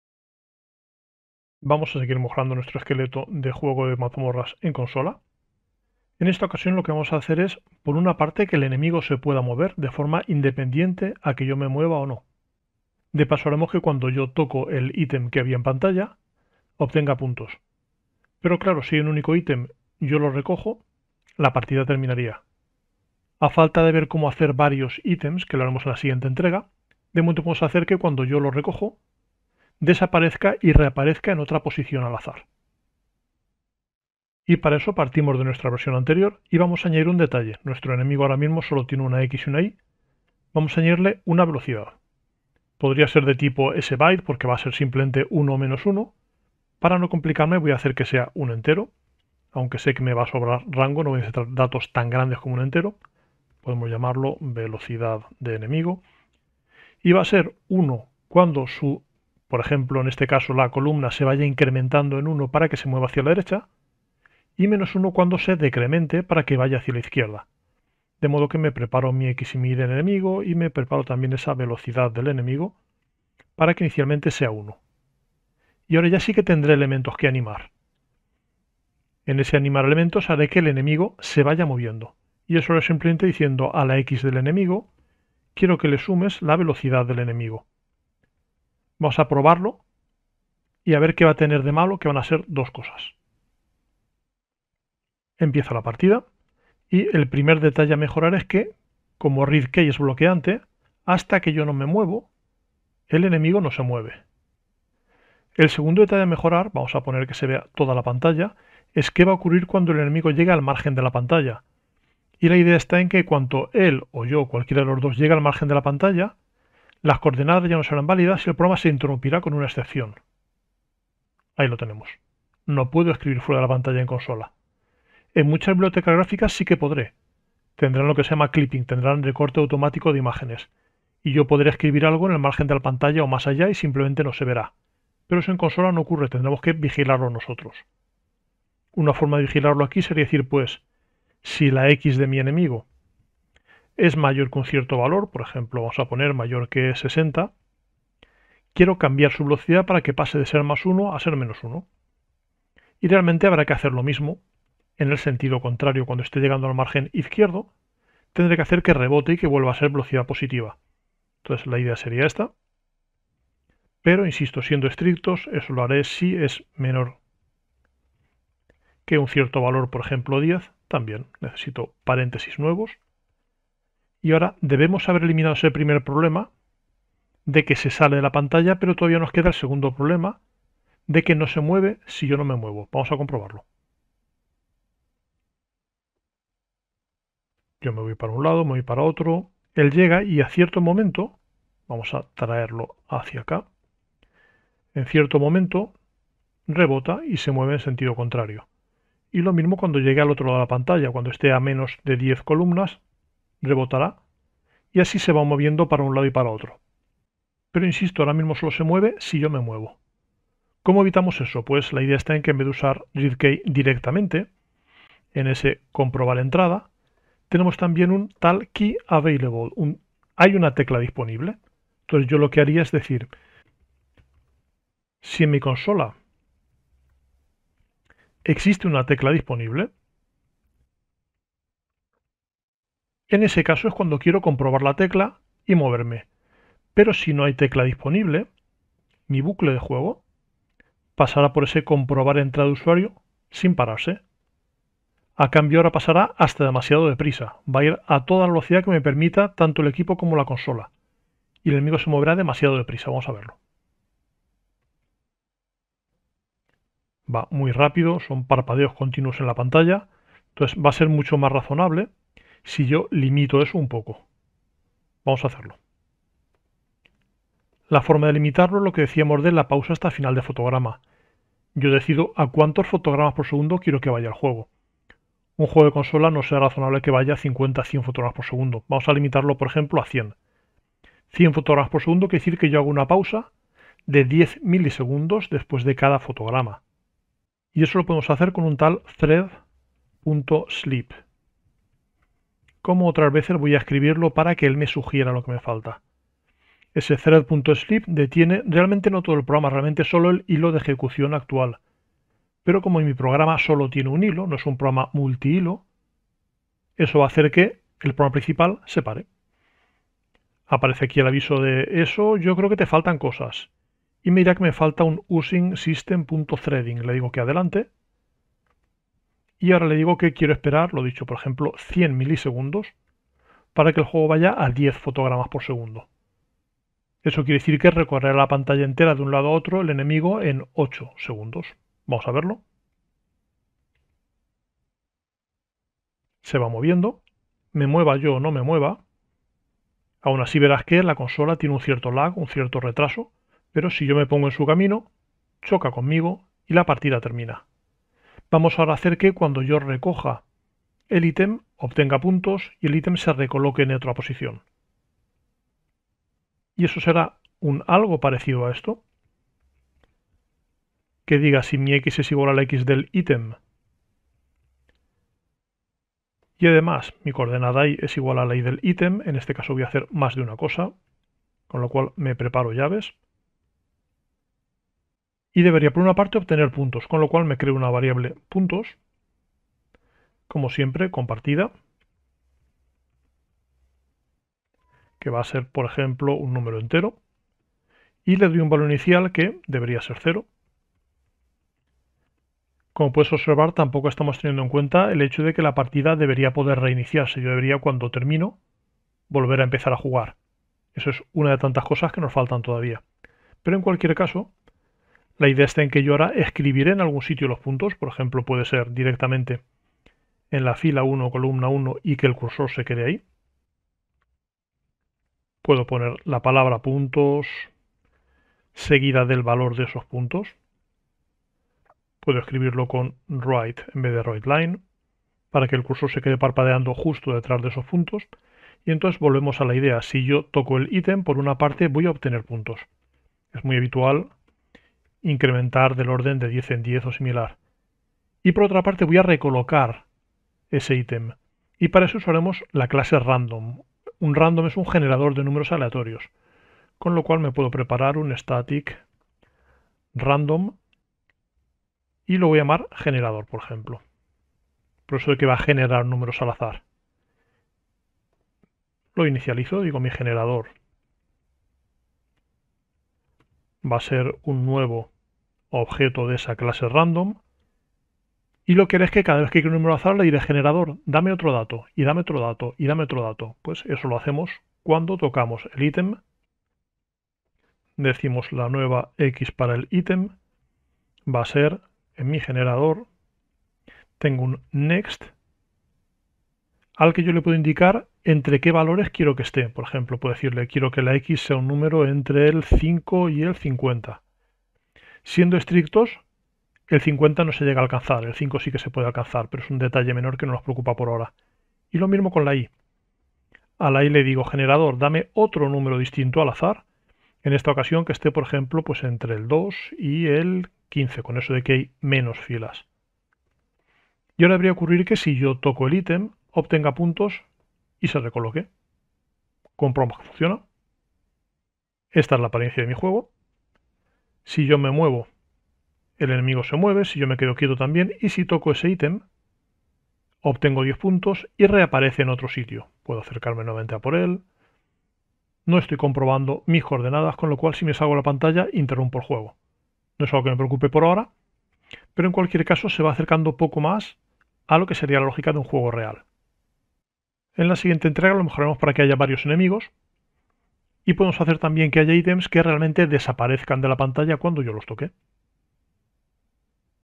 Movimiento simultáneo - Juego de "mazmorras" en consola, parte 2 - Juegos sencillos en C# 05b
Juegos sencillos en C# 05a - Juego de "mazmorras" en consola, parte 2: movimiento simultáneo, puntos, azar.

Contenidos:
00:00 Presentación
00:34 Añadiendo una "velocidad" al enemigo
01:15 Cambio en "AnimarElementos" y problemas pendientes
02:15 Rebote de enemigo en los márgenes laterales
03:25 No bloquear movimiento: Console.KeyAvailable
04:16 Pausa hasta final de fotograma: 10 fps con Thread.Sleep
05:47 Puntos al recoger un "ítem"
07:16 Recolocar el "ítem" en otra posición al azar
09:07 Previsión para la próxima entrega

Si no sabes nada de C#, quizá este curso gratuito de 2 horas te ayude: